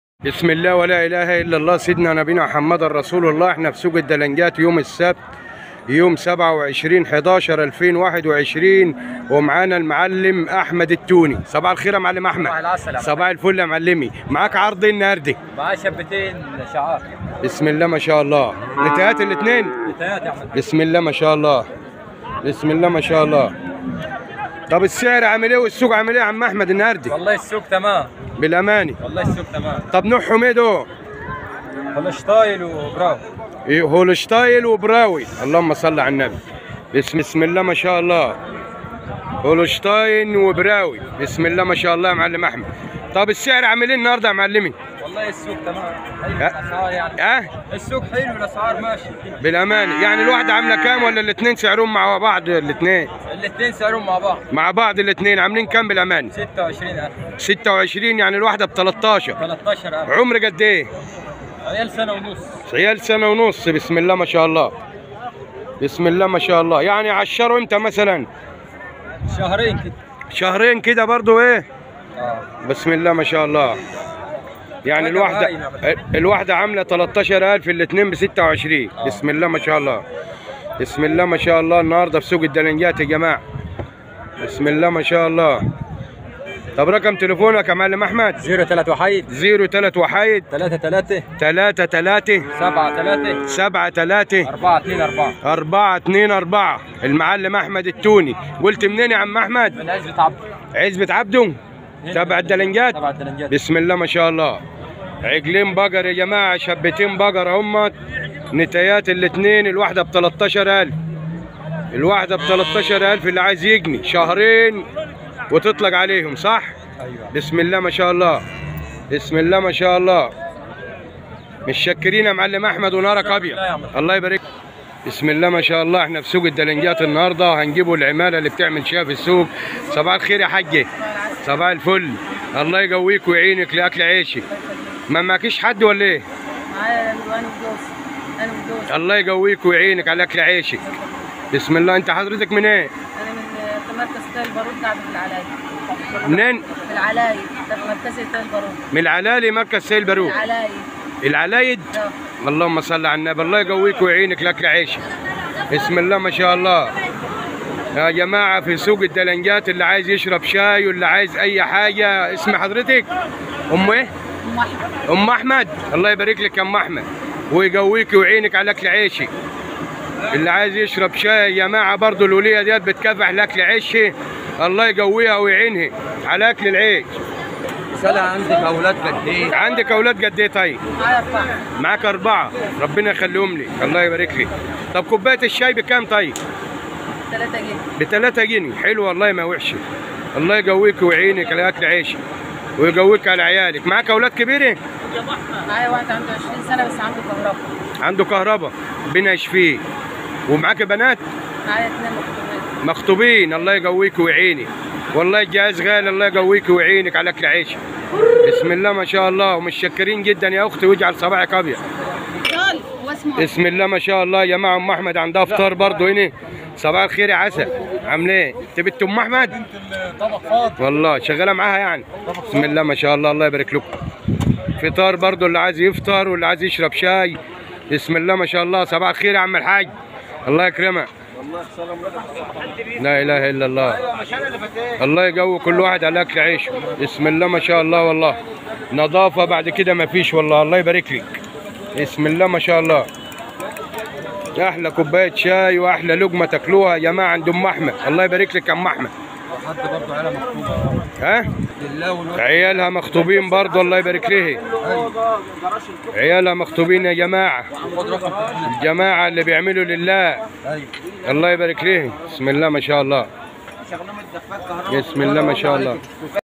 أسعار الشعاري البقر في سوق الدلنجات للمواشي يوم السبت بتاريخ ٢٠٢١/١١/٢٧
اهلا وسهلا بكم
متابعين قناة المواشي اليوم
سنقدم لكم
يوم السبت أسعار سوق الدلنجات
يوم الاحد أسعار سوق دمنهور العمومي  ويوم الثلاثاء أسعار سوق أبو حمص
يوم الاربعاء أسعار سوق التوفيقية
يوم الجمعة أسعار سوق أبو المطامير